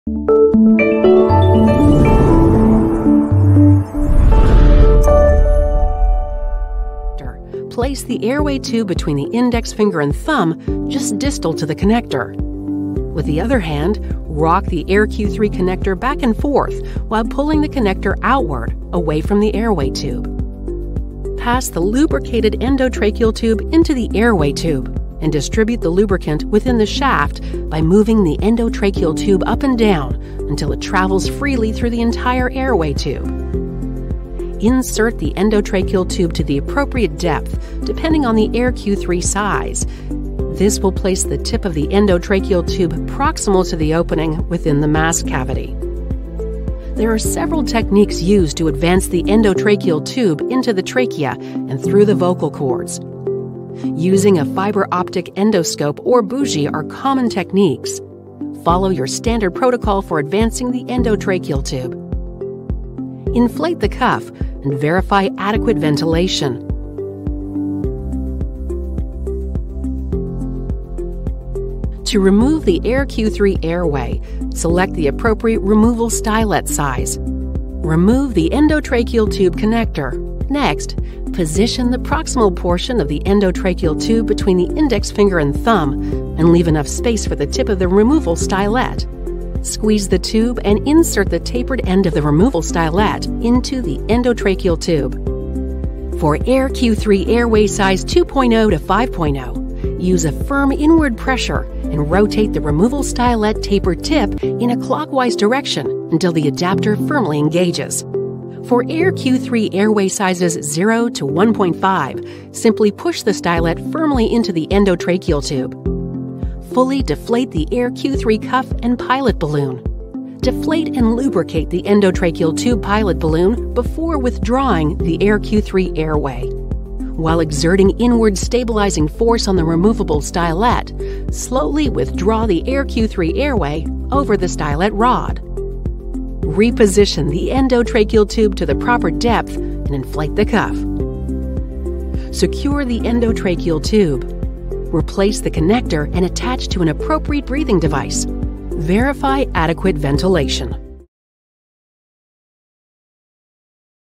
Air Q Removal Stylet Feature
The Air Q3 intubating supraglottic airway device has an optional removal stylet feature which allows for the 'railroad' of an ETT.